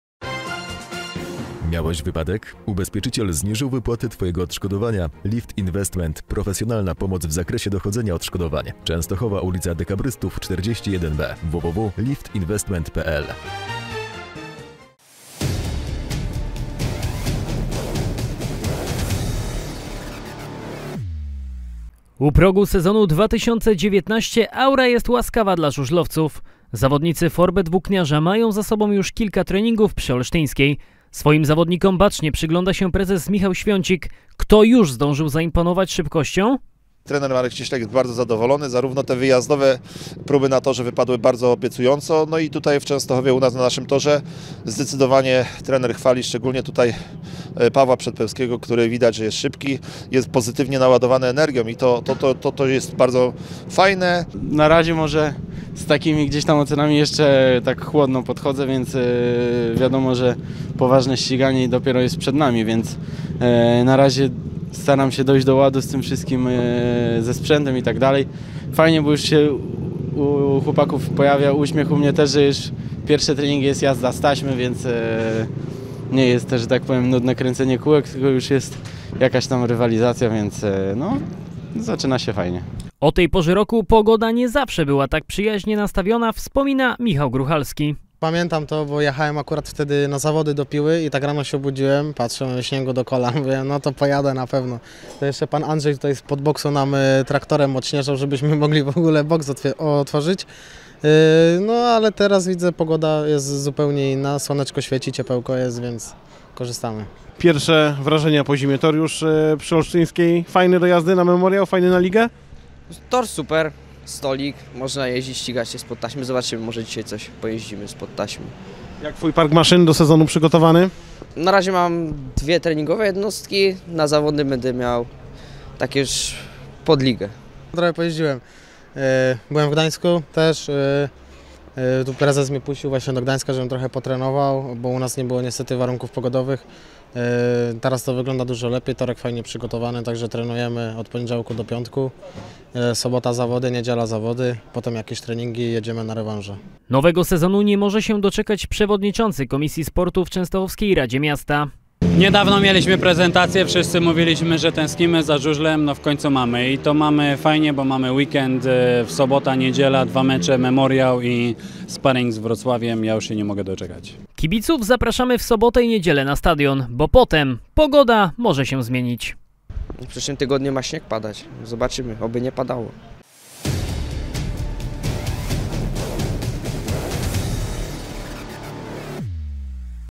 Lwy już szarżują. Byliśmy na treningu - VIDEO
Żużlowcy forBET Włókniarza Częstochowa od minionego poniedziałku trenują na własnym obiekcie. W dotychczasowych treningach brakowało tylko Leona Madsena oraz Fredrika Lindgrena. Na jednym z nich pojawiła się natomiast ekipa Radia FON, która pytała o pierwsze spostrzeżenia zawodników.